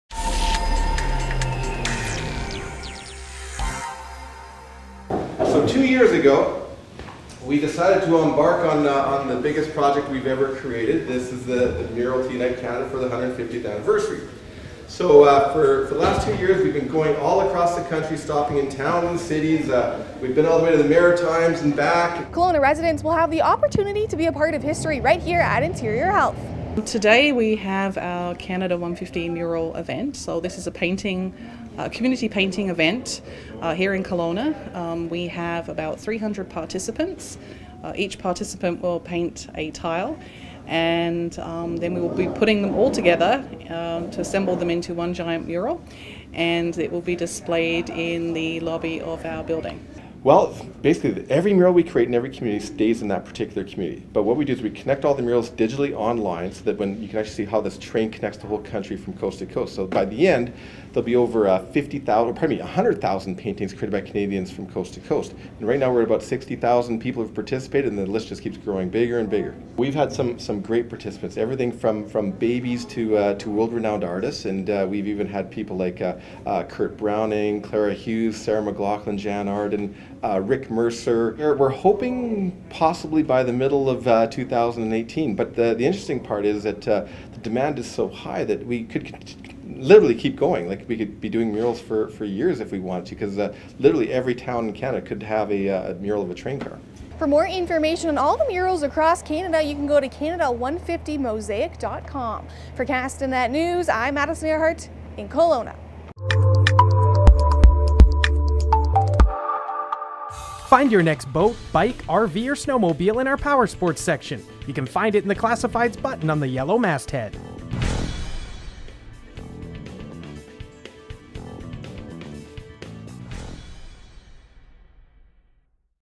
History in the making
Be a part of a Kelowna mural, and your art will hang in Interior Health's new Community Health & Service Centre.